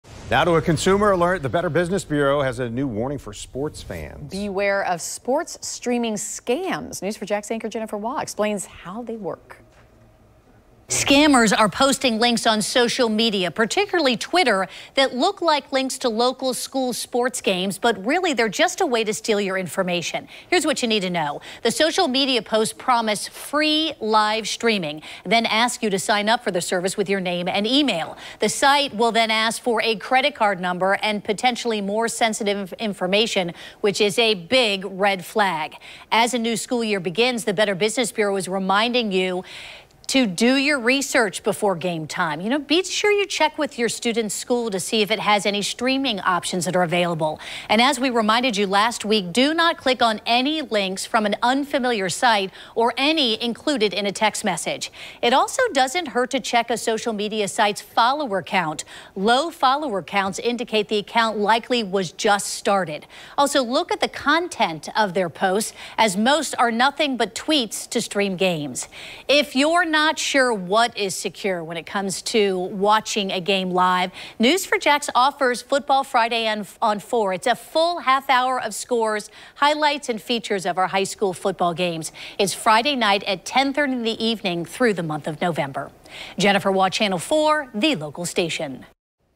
BBB warns of sports streaming scams
The Better Business Bureau is warning sports fans to be aware of sports streaming scams.